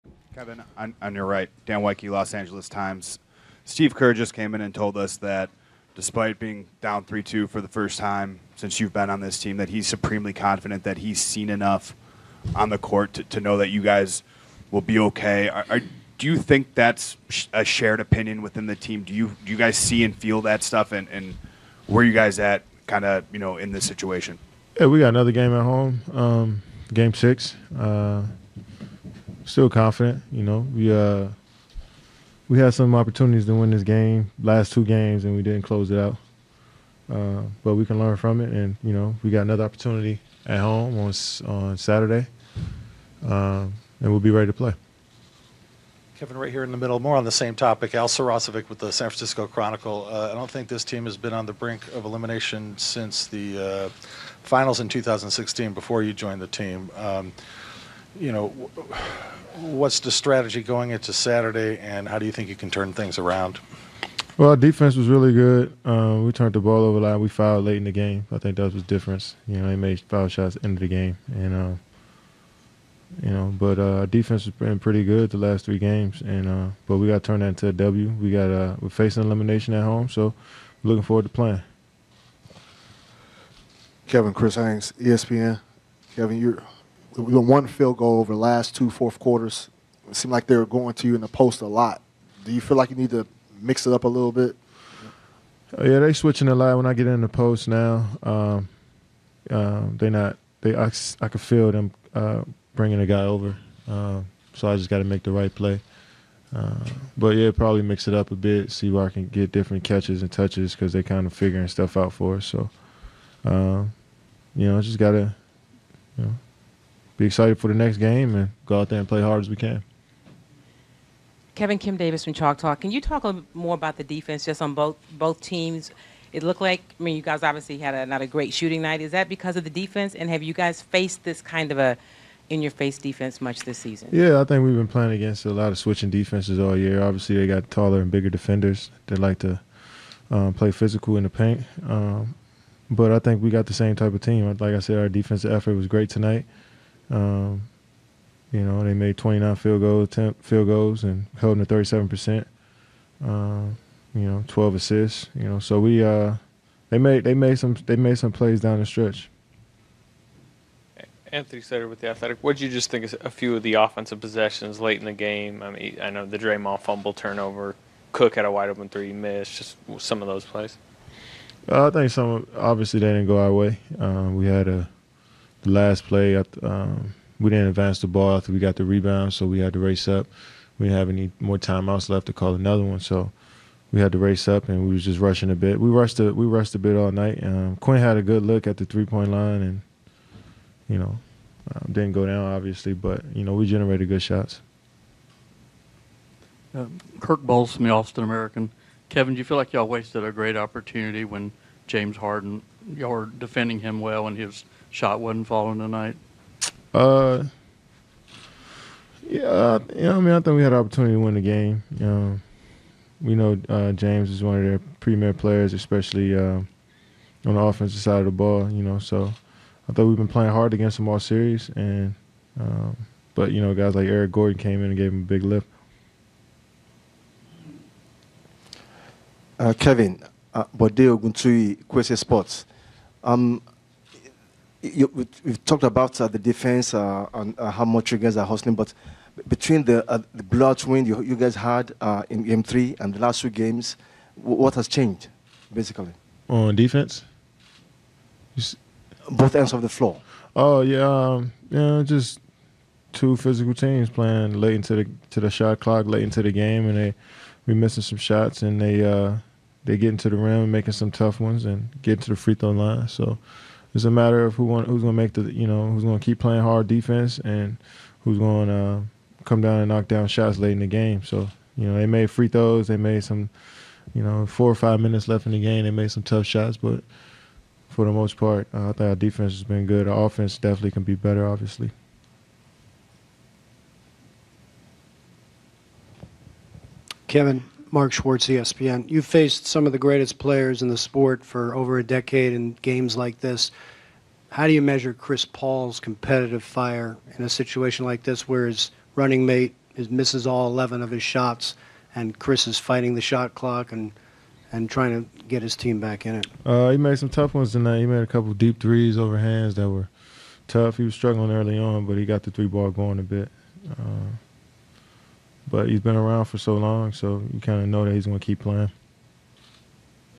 Kevin Durant | Game 5 Western Conference Finals Press Conference
"Still confident. We had some opportunities to win this game and we didn't close them out." KD reflects on the Warriors 94 - 98 loss on the road to the Houston Rockets. Durant led the team with 29 points.